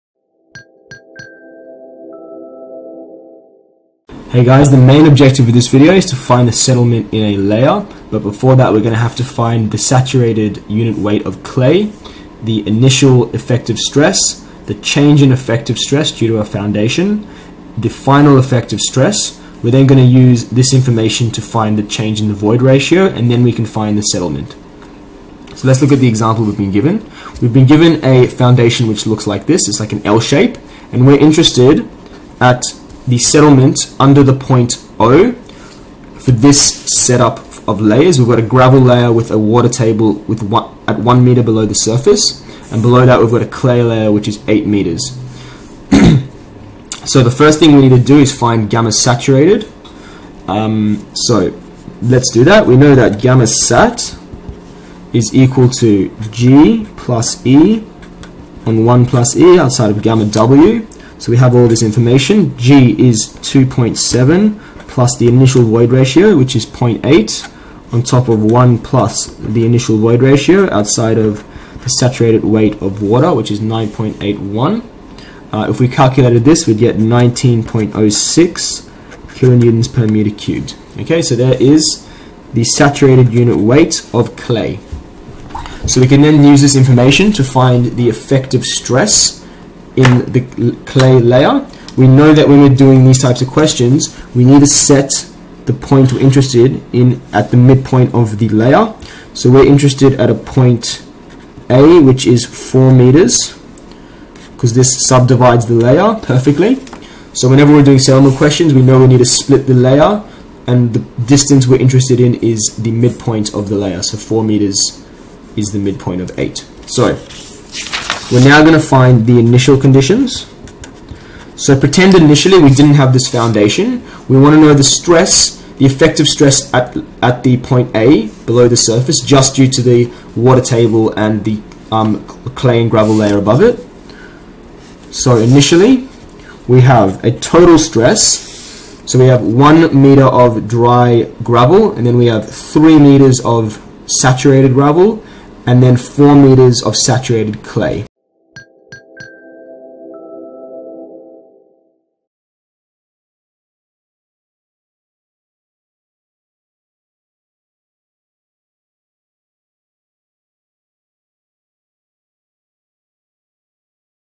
One Dimensional Settlement Example Part 1 | Soil Mechanics
For more FREE video tutorials covering One Dimensional Settlement of Soils and Soil Mechanics.

In this video we apply the knowledge from this topic to calculate the stress increase under an L shaped foundation in order to find the final settlement in the soil section. By finding the unit weights of the soil layers and using an influence factor graph we can calculate the total change in stress and thus the final stress value.

This video is part of the topic One Dimensional Settlement of Soils where the knowledge from the previous topic is applied in determining the amount of settlement in a one dimensional soil context. The settlement level is closely linked to the voids ratio and applied stresses which are calculated for different foundation types.